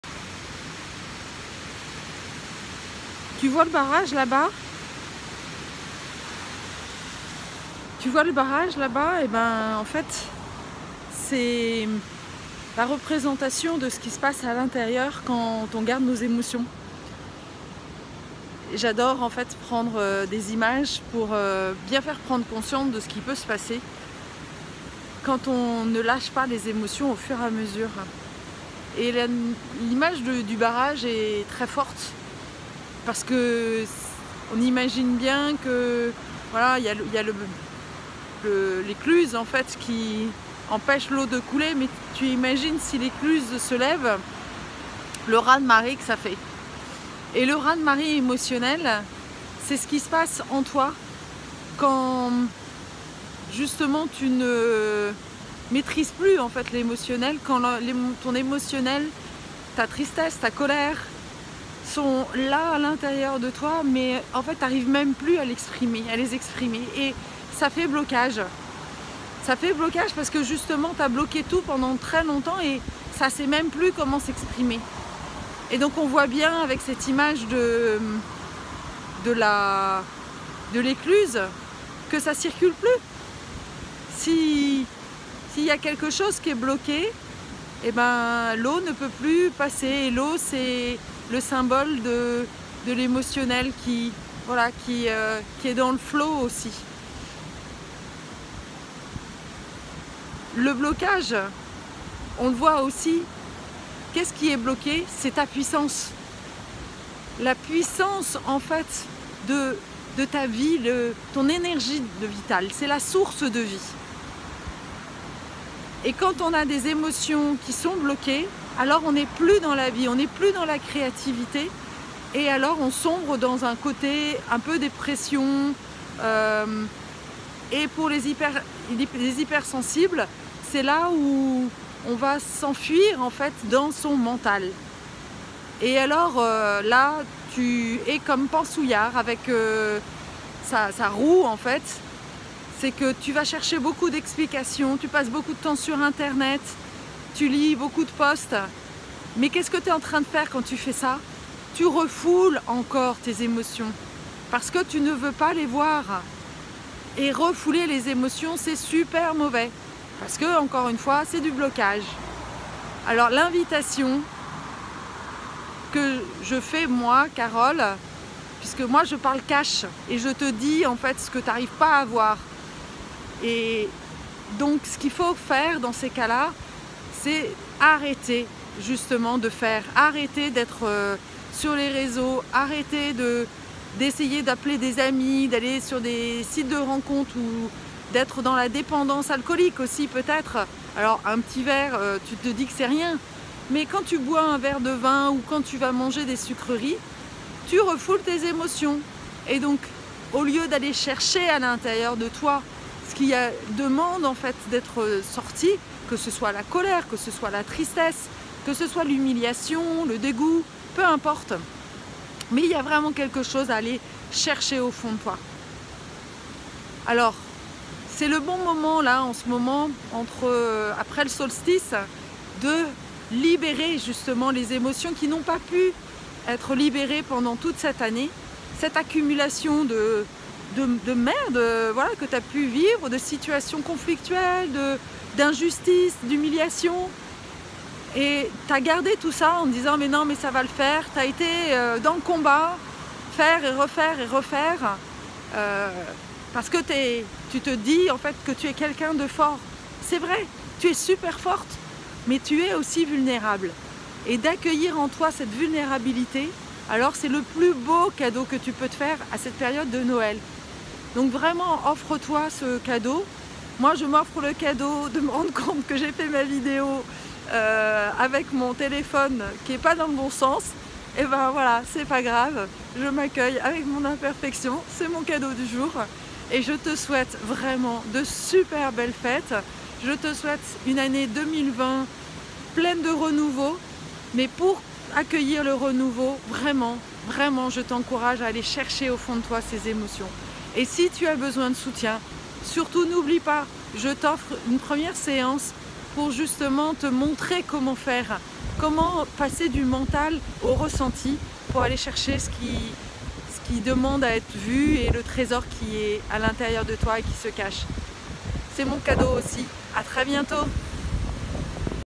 Transformer les merdes 2019 comme terreau pour 2020 ☀️🔥
Le solstice est la meilleure période pour se libérer des pbs accumulés , les transformer et créer une année 2020 lumineuse et pleine d’abondance ⛅️⚡️